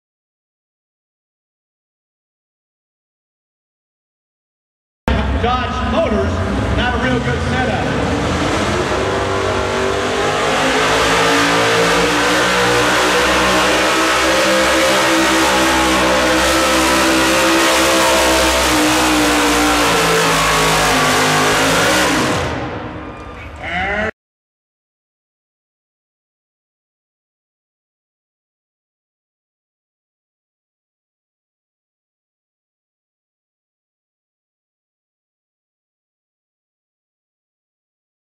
Power Wagon-Western Farm Show-2/20/15
Matt Strange pulling Power Wagon at the Western Farm Show in Kansas City, MO